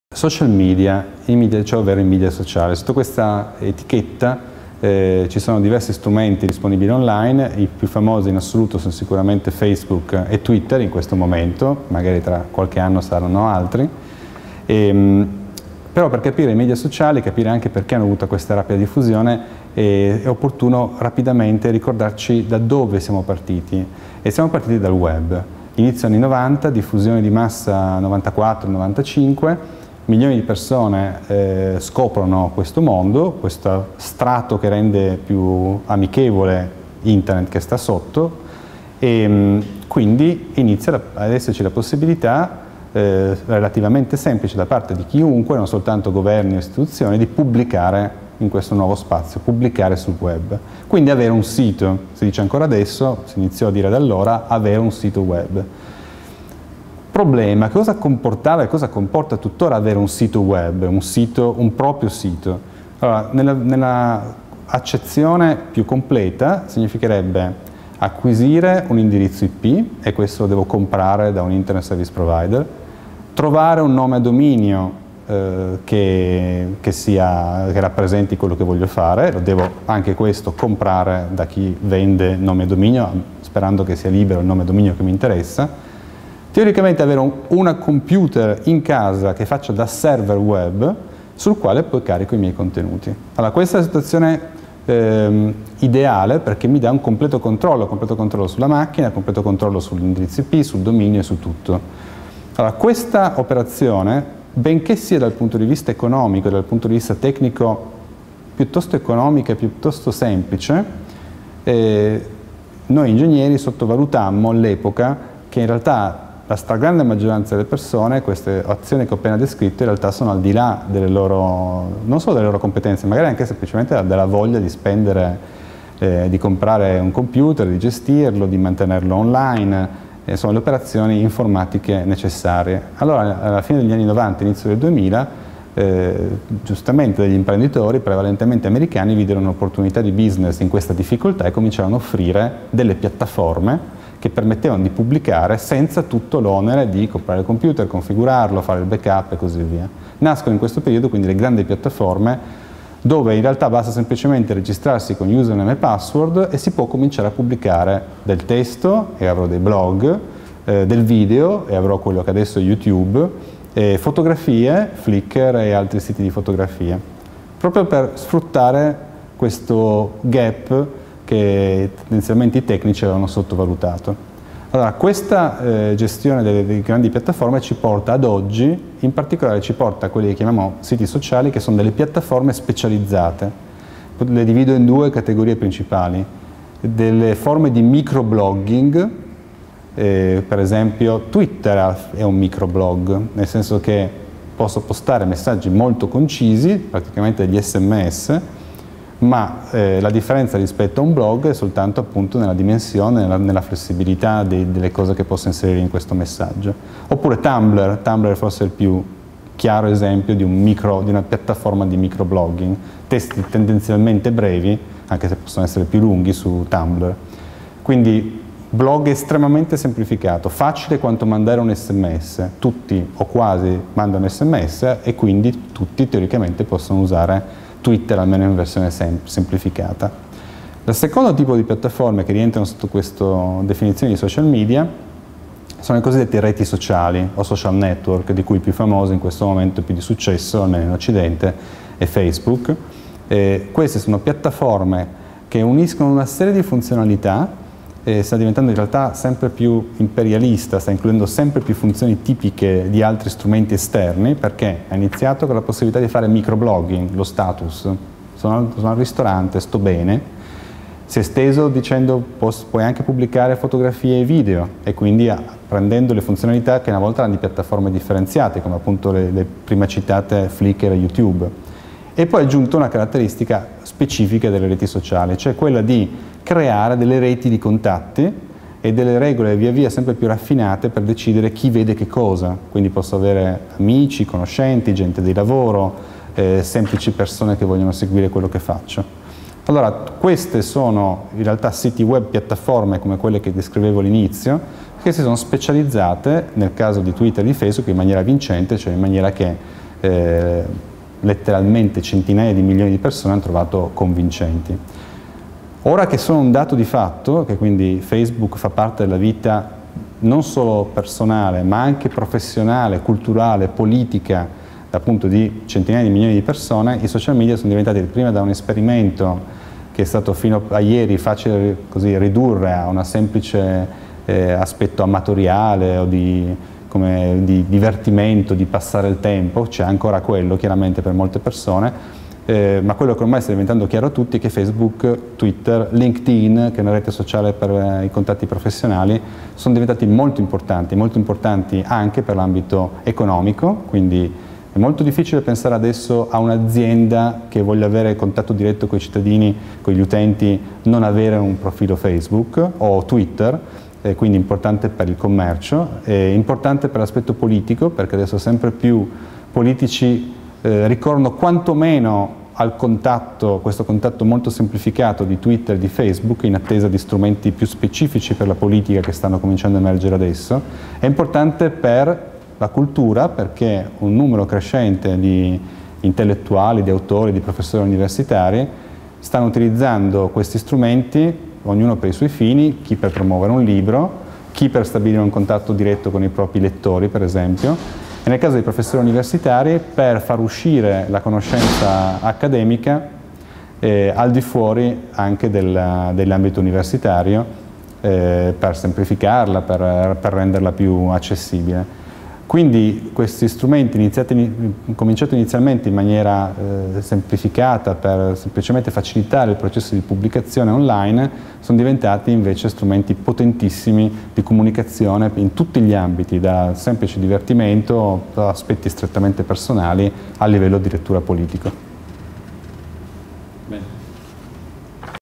Social media: origini e caratteristiche di un fenomeno della Rete
Juan Carlos De Martin, professore di ingegneria informatica al Politecnico di Torino, spiega le ragioni del successo dei social media a partire dalle origini, con la diffusione dei siti Web, che hanno dato a tutti l'opportunità di conquistare uno spazio in Rete. I social media si articolano in due grandi categorie: reti sociali e microblogging. Oggi sono utilizzati in vari campi, dall'economia alla politica, dalla cultura alla vita quotidiana.